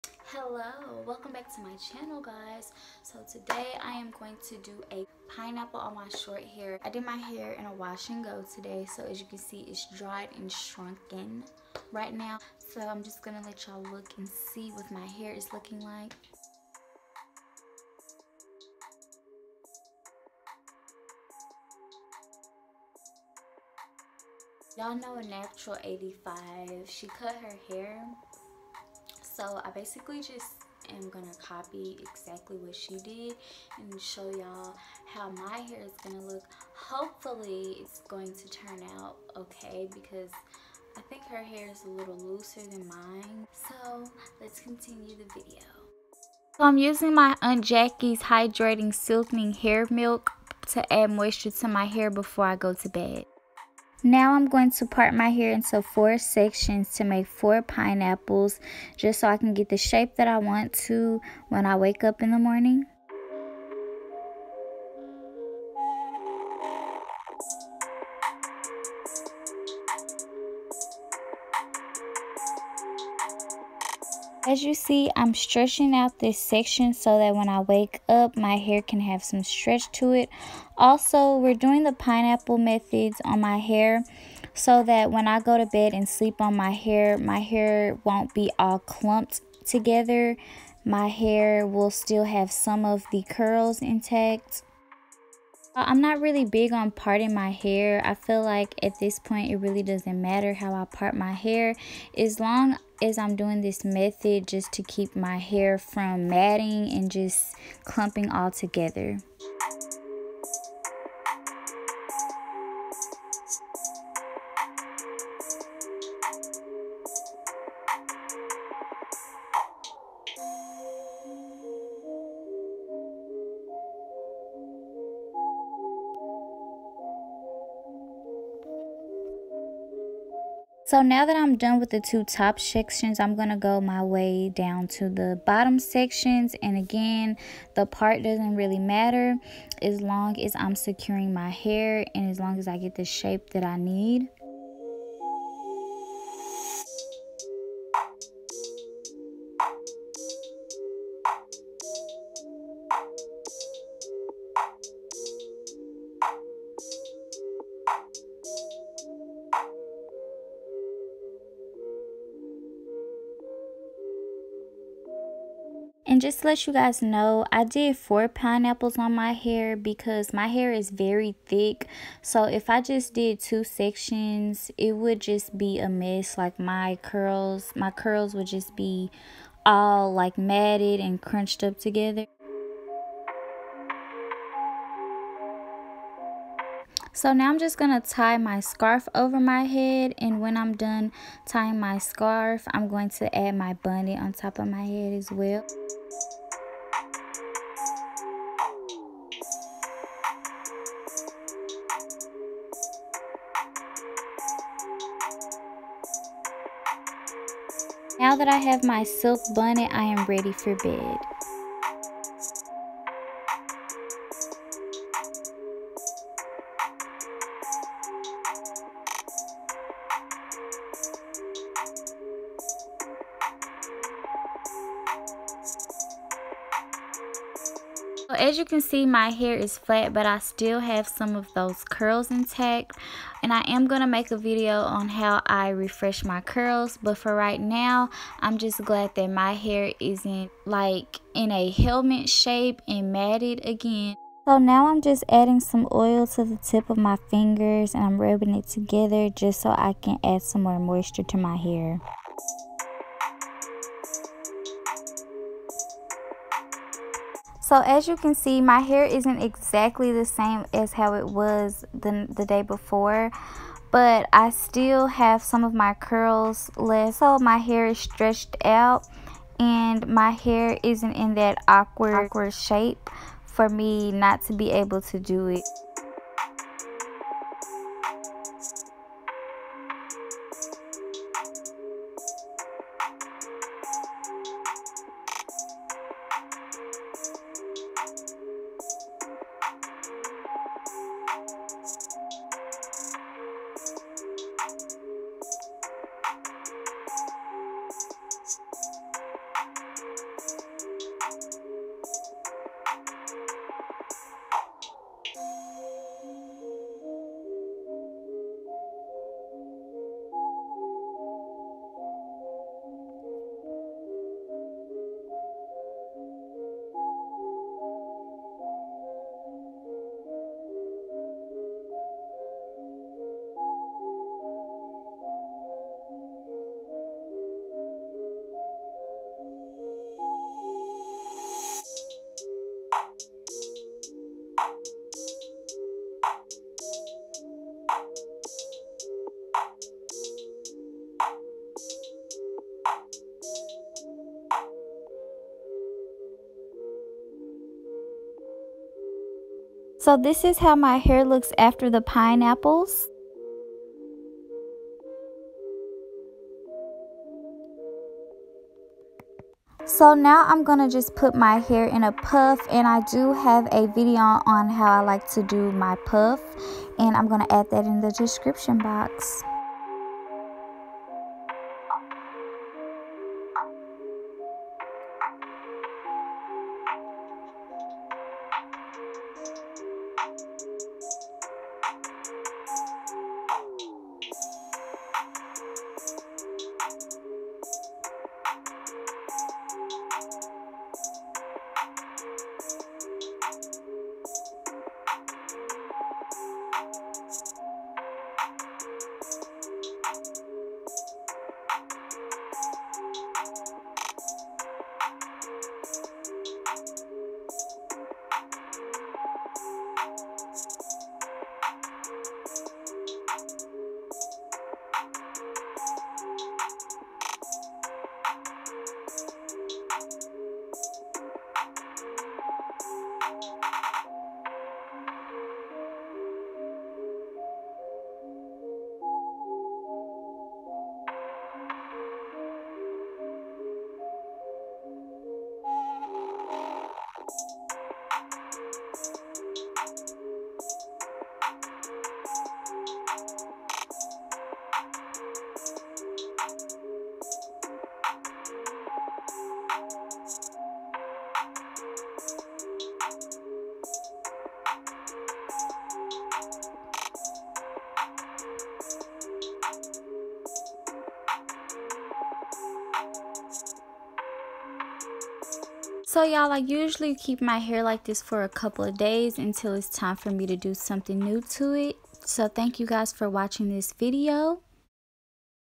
How to Pineapple Short 4C Natural Hair| Night Routine
Video Creds: Naptural85

Hey y’all, I seen Naptural85 pineapple her shirt hair and wanted to try it on mines. Hope you guys enjoy this video and try this on your hair!